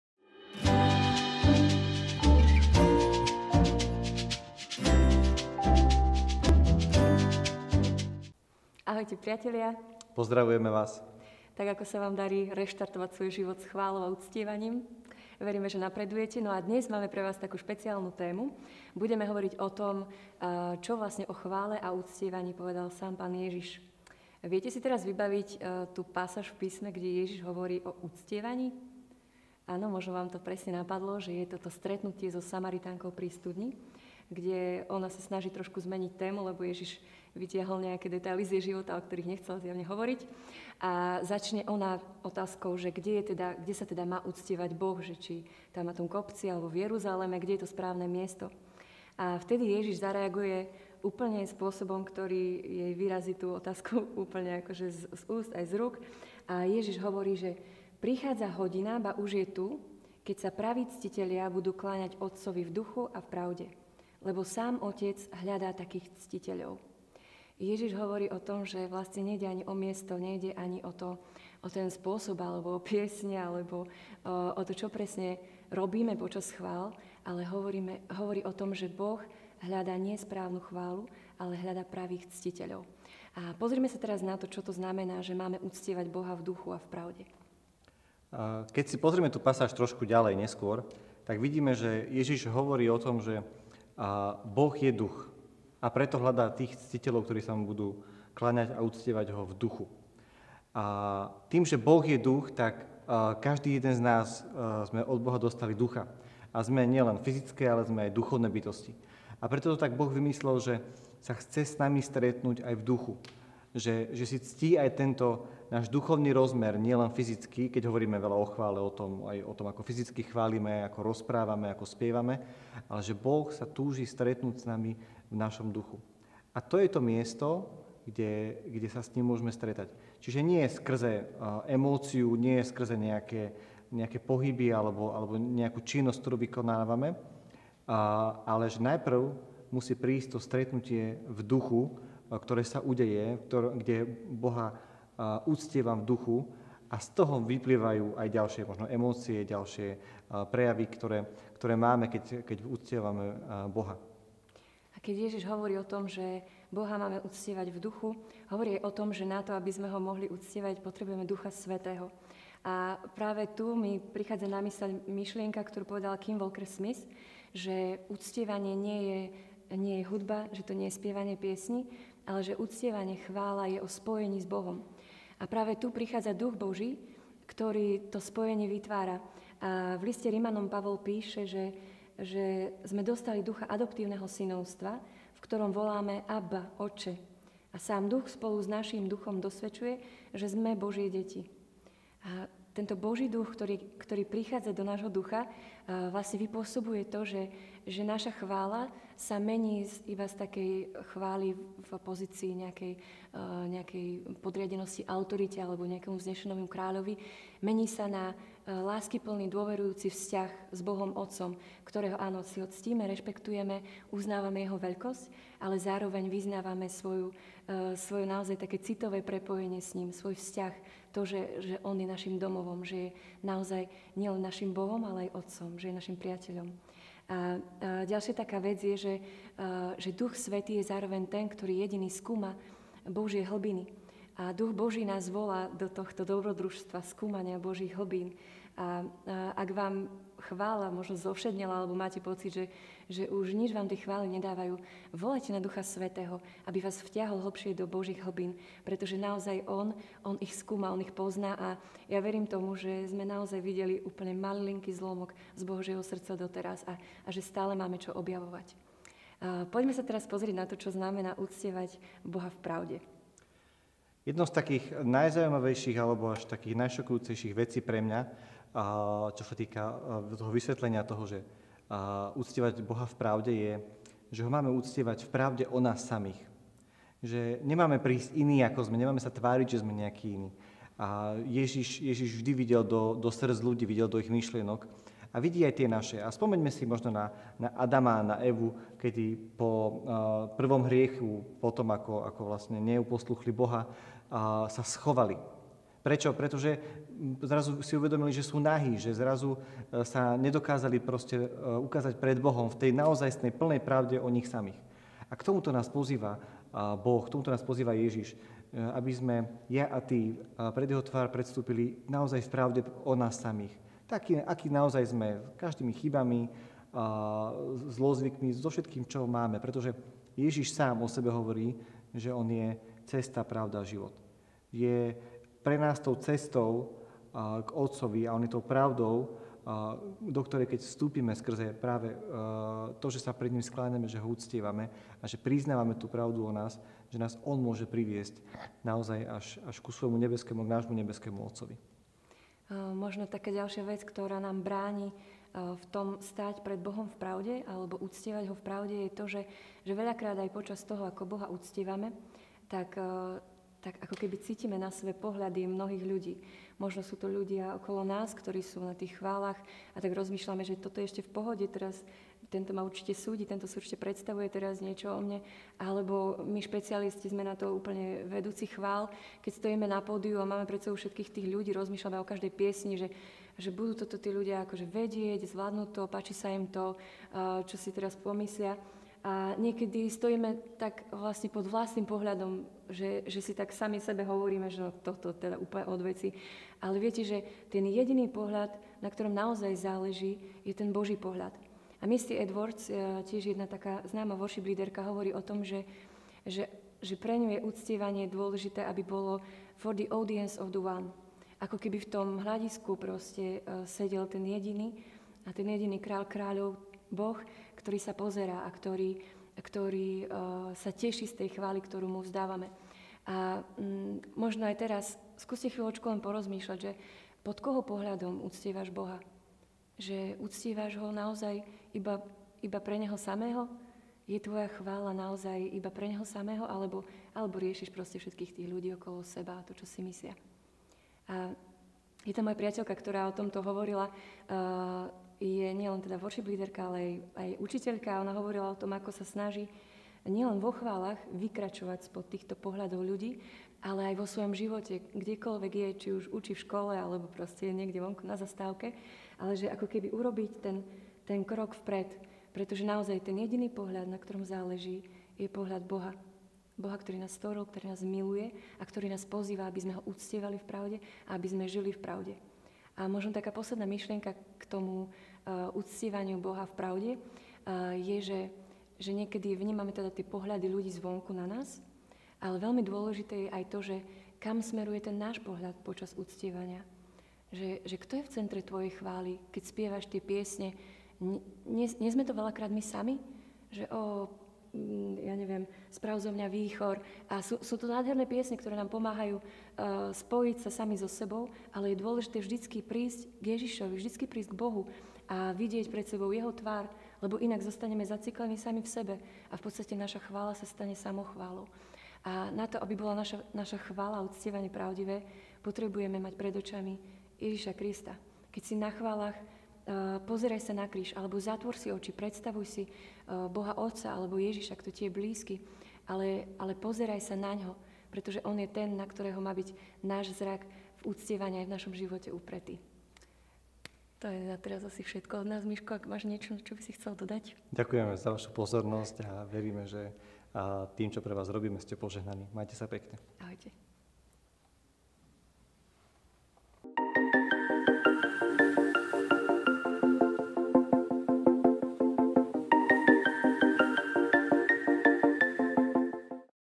Klaňanie sa Bohu v Duchu a v Pravde
Misiefilmom.sk vám prináša ďalšiu časť zamyslenia na tému Reštartuj svoj život s chválou od spoločenstva pri Dóme sv. Martina v Bratislave, ktoré boli natočené v spolupráci s nami. Michal Steiner spolu s Evkou Hreškovou sa spolu zamýšľajú nad tým čo to znamená klaňať sa Bohu v Duchu a v Pravde.